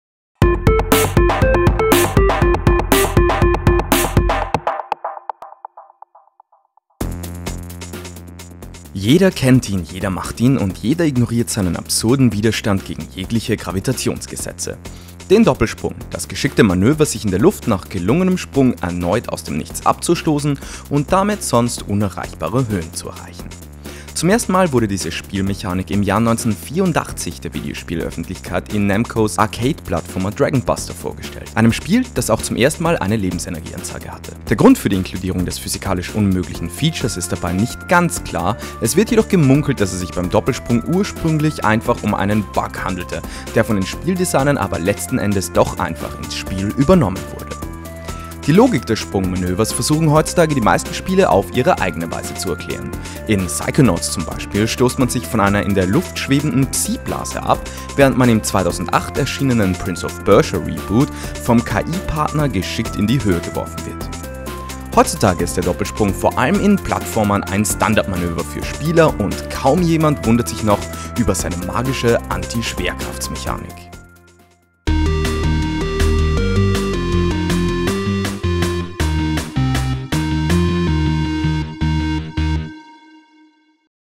Woher kommt eigentlich... der Doppelsprung?
In diesem Videospecial gehen wir kurz und knapp dem Ursprung und der Geschichte einer der bekanntesten Spielmechaniken der Gamingwelt auf den Grund: Dem Doppelsprung.